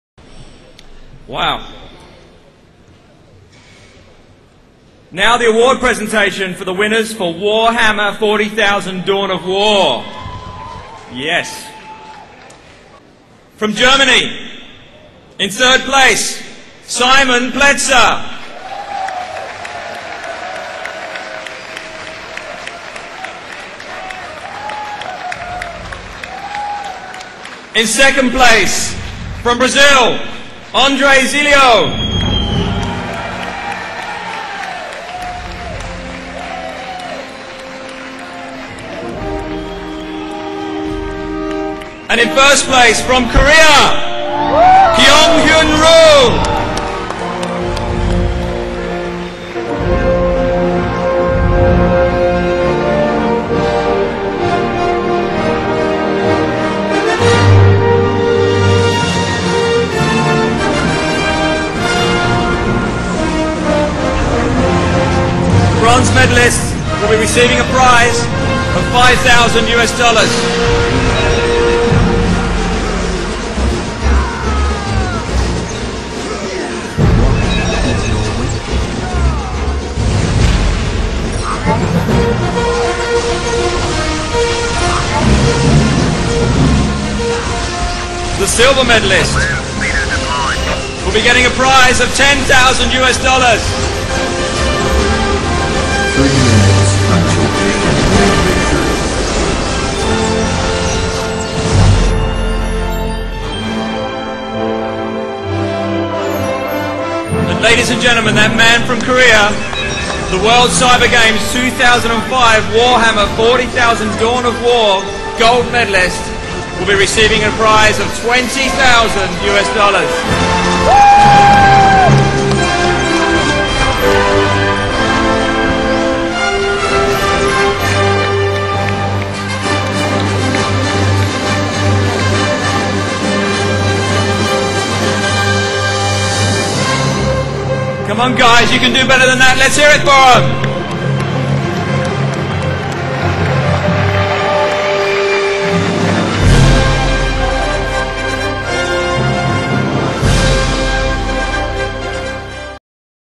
WCG 2005 Prize Ceremony for Warhammer 40k Dawn of War
Myself at WCG 2005 Prize Ceremony